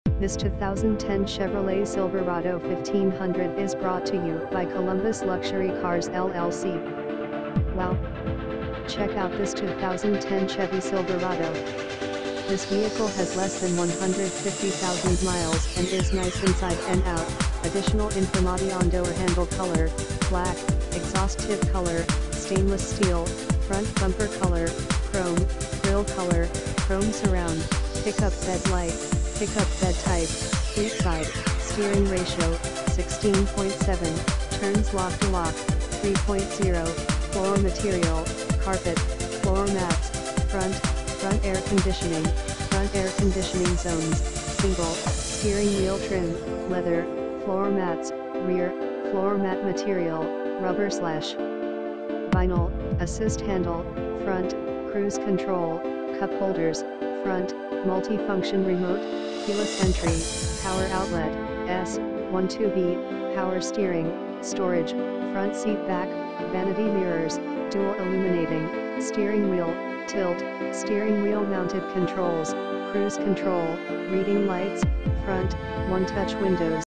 2010 Chevrolet Silverado 1500 4WD Ext Cab 143.5" LT (Columbus, Ohio)
4880 Sinclair RD
Columbus